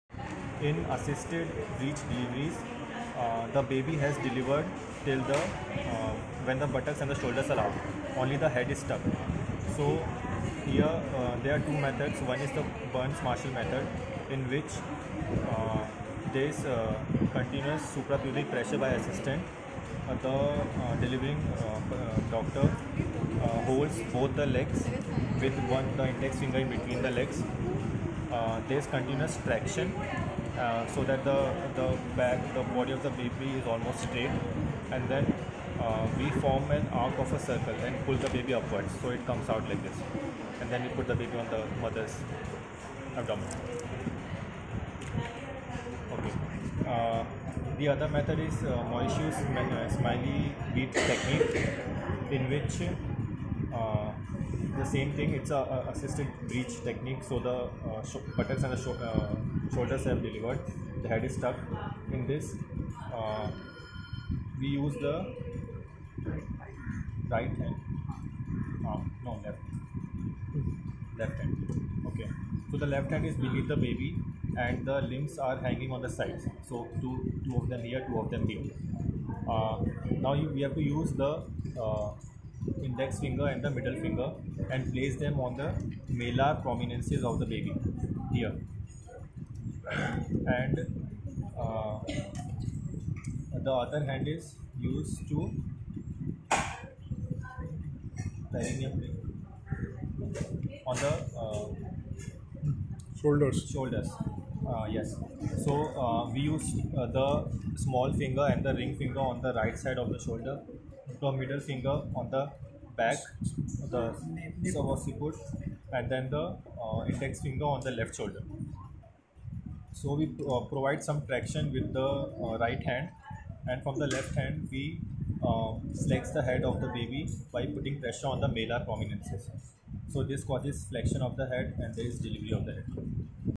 Assisted breech delivery | Burns Marshall method | mauriceau smellie veit technique
This video is about the Burns Marshall technique and Mauriceau Smellie Veit technique for Assisted vaginal breech deliveries.